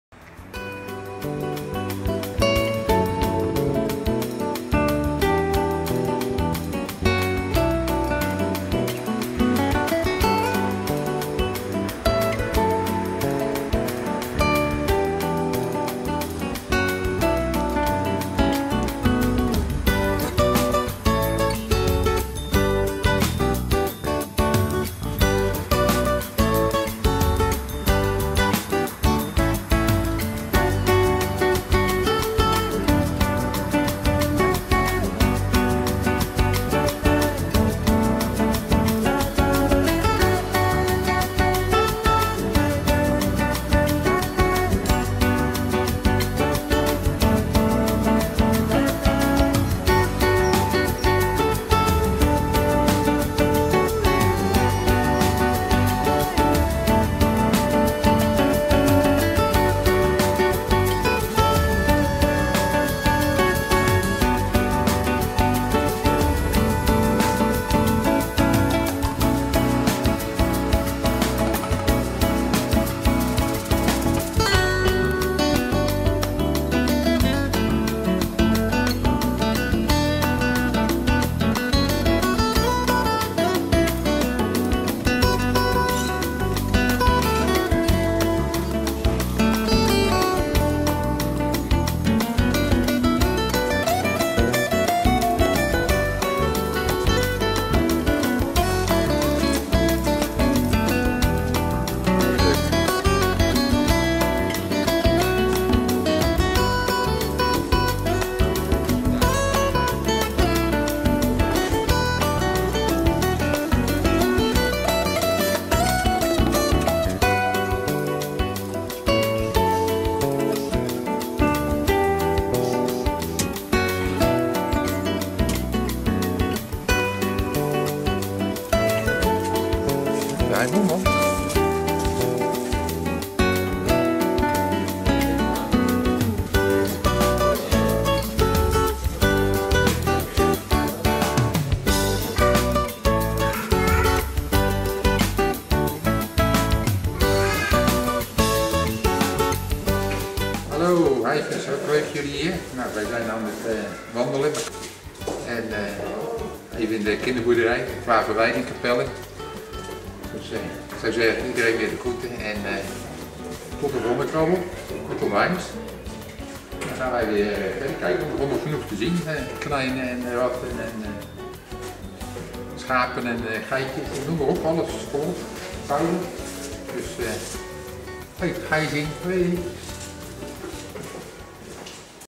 Kinderboerderij: LuckyWim,s Hyvesgroeten 25
LuckyWim bezoekt de kinderboerderij Klaverweide in Capelle aan den IJssel. deel 1